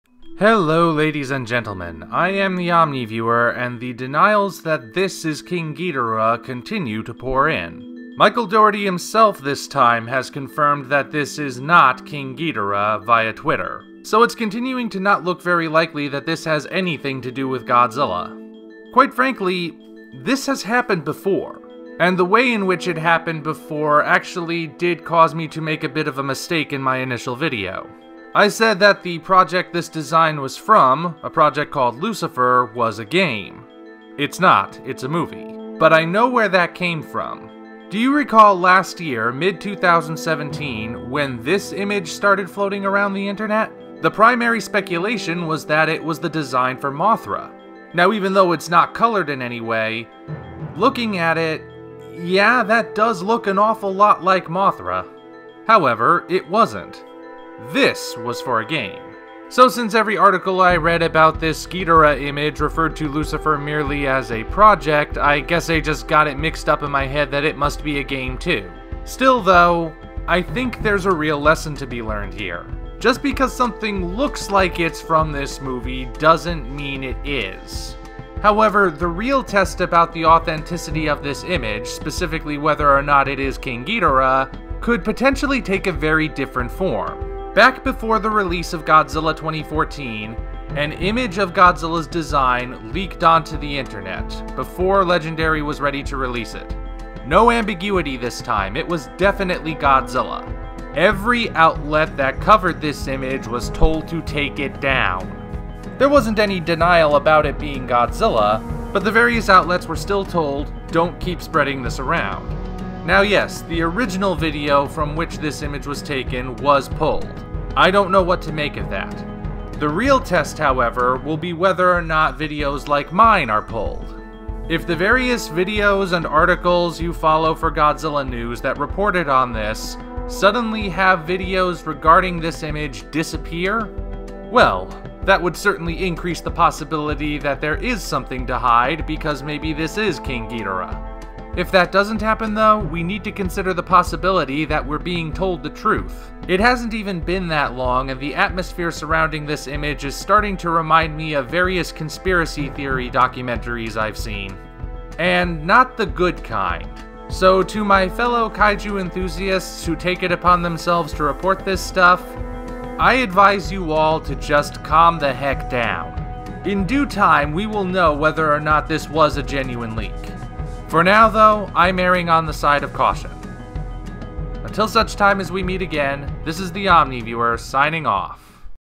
Further Word on the Alleged King Ghidorah Leak
Not only has Mike Dougherty come forward, but the Omni Viewer brings a reminder from the past.